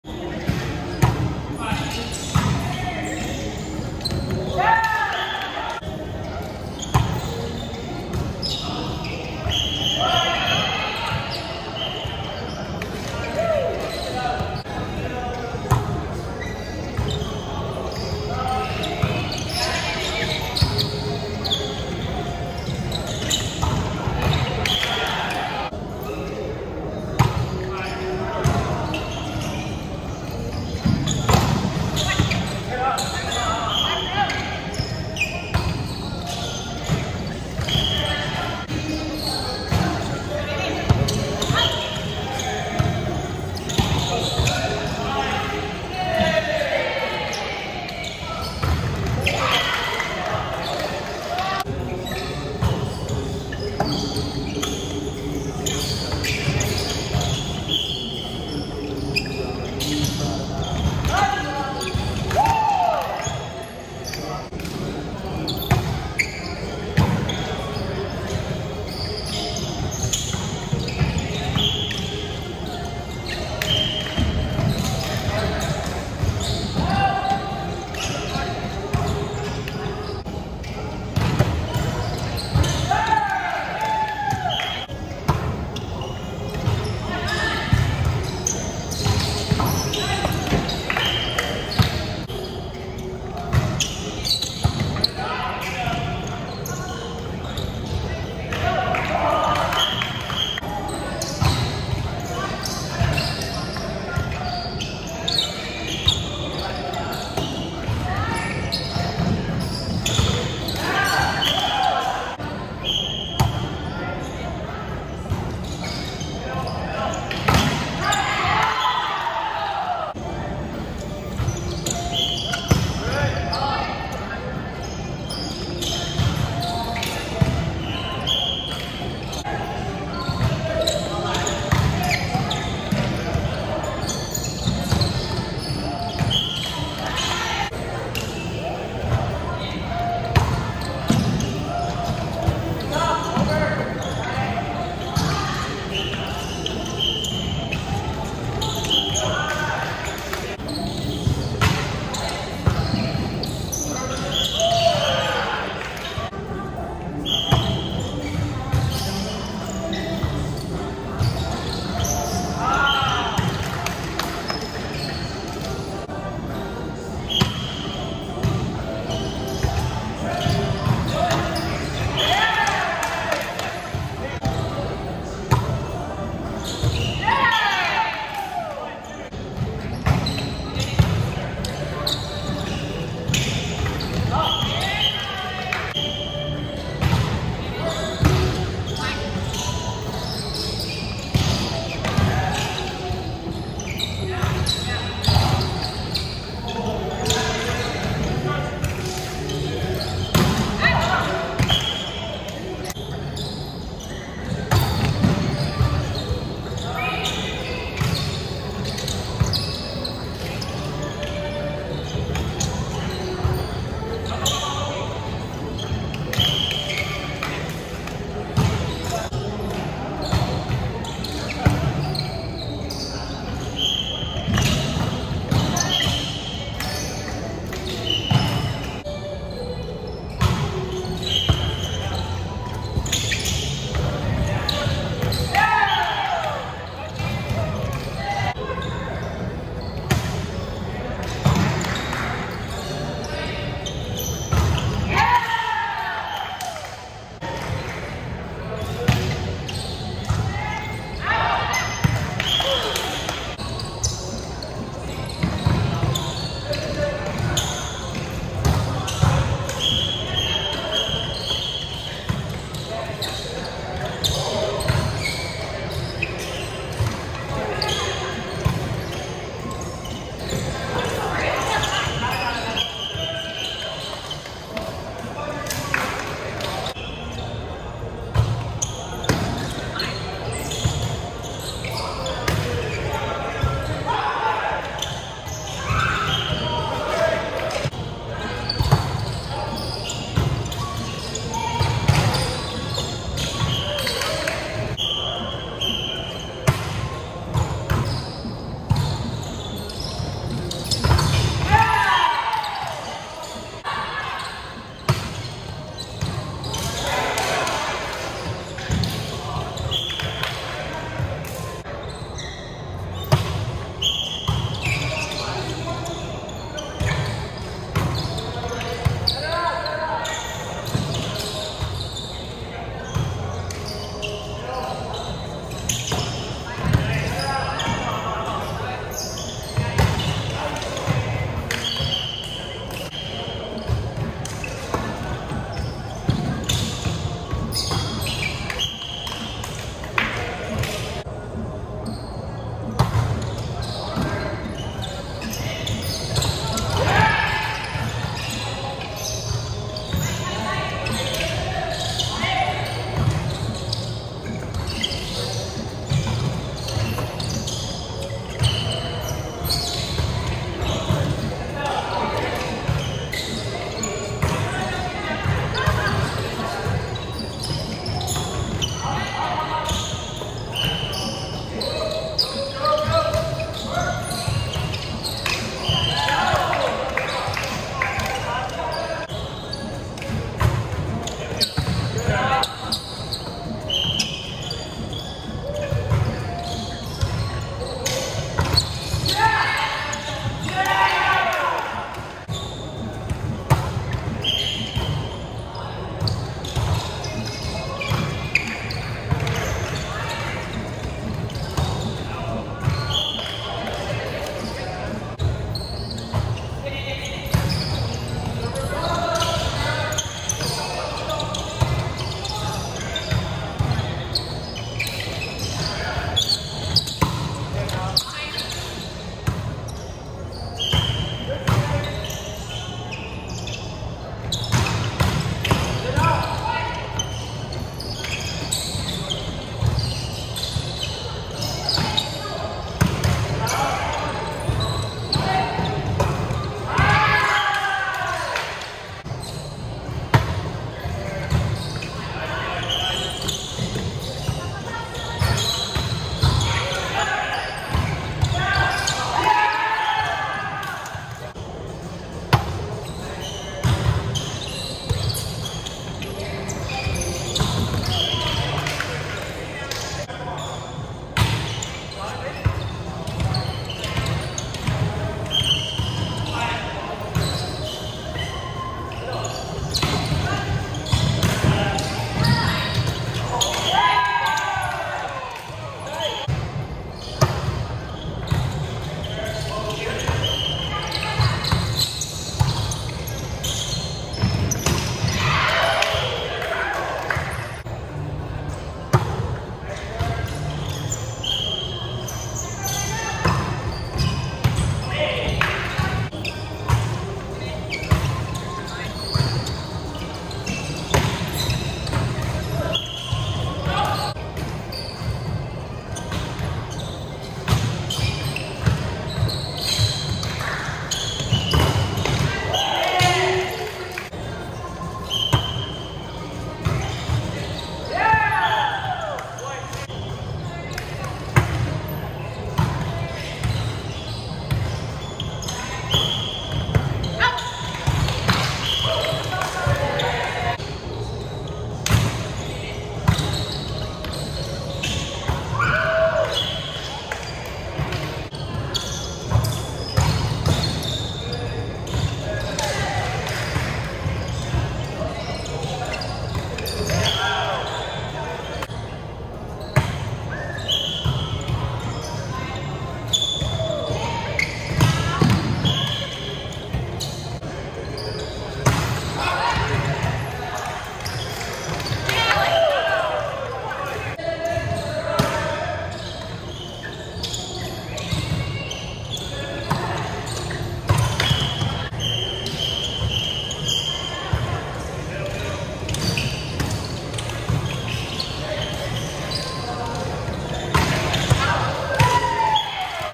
6/26/2023 BSSC LowINT Monday Volleyball League Beef Tataki vs. KatsuDon
21-13 Win
20-19 Win